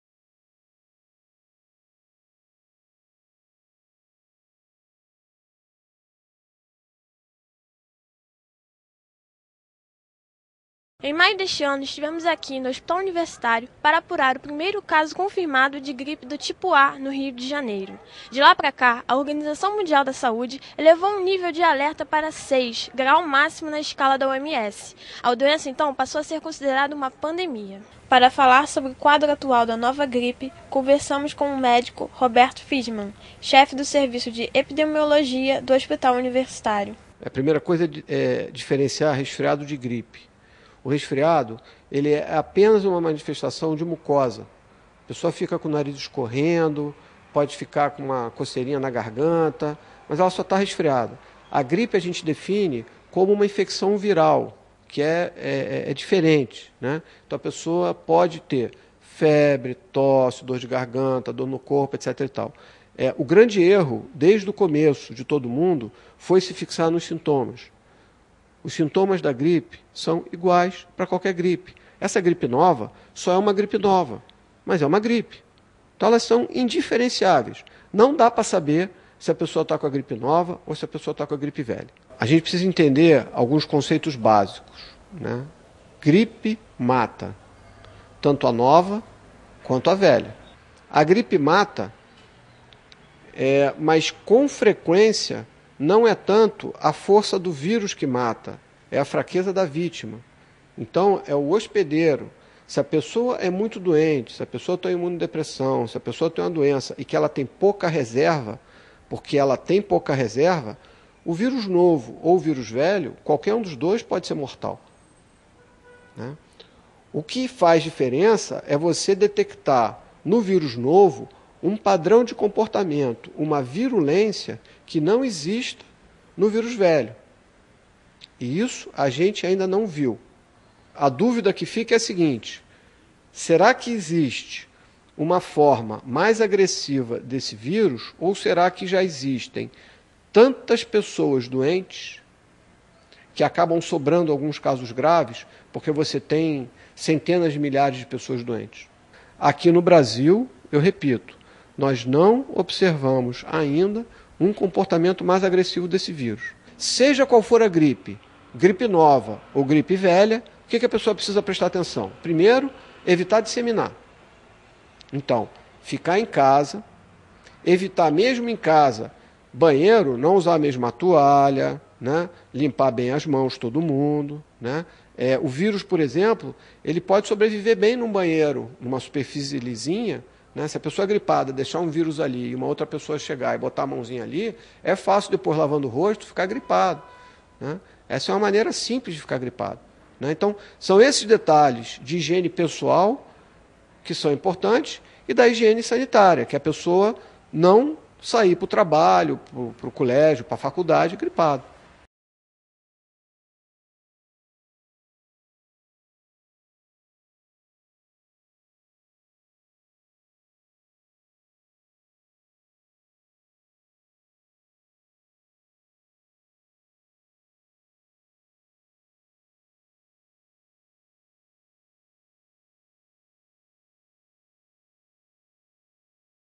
17/07/2009 - Gripe tipo A
Rio, 17 de julho de 2009

Gripe tipo A

Em maio deste ano foi detectado o primeiro caso confirmado de gripe tipo A no Rio de Janeiro. De lá para cá o quadro da doença evoluiu e hoje é considerada pandemia pela Organização Mundial da Saúde.

Confira a entrevista que nossa equipe fez com o pneumologista do Hospital Universitário Clementino Fraga Filho.